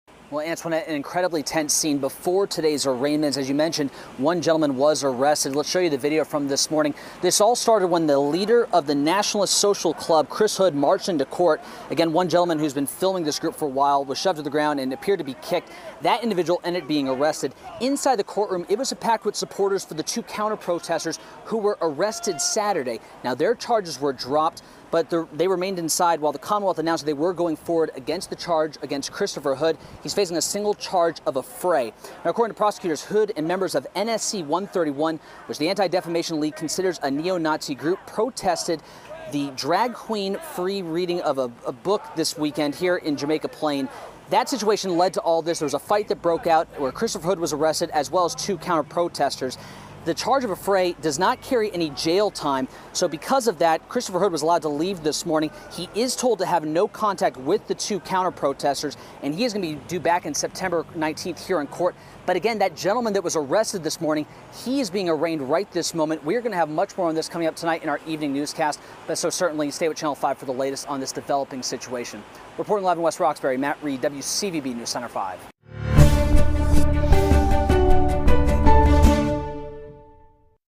Scuffle outside court as neo-Nazi faces charges
The founder of a neo-Nazi group was charged Monday after a protest in the Jamaica Plain neighborhood of Boston during an LGBTQ meeting during the weekend.